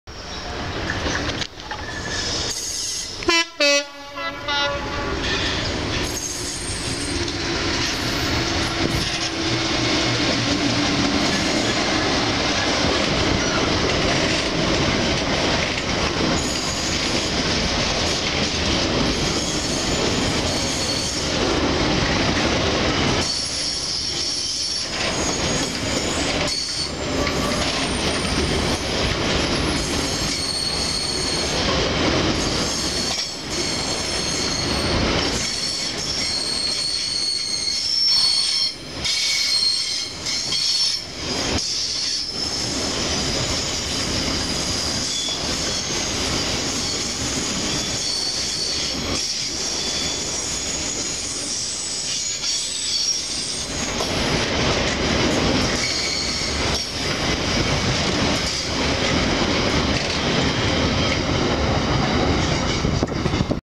66070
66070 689J 0725 Wilton Efw Terminal to Knowsley Freight Terminal with Ian Burrows at the helm